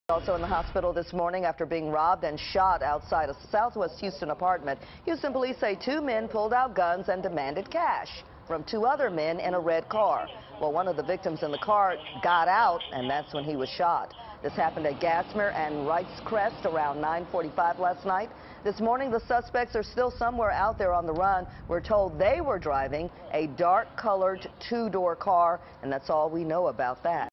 HPD: Man shot during robbery outside SW Houston apartment
A man was hospitalized after being robbed and shot outside a southwest Houston apartment, police said.

KHOU 11 News,Houston,Texas,Houston videos,Texas videos,news videos,weather,sports,Houston news,featured video,travel,weather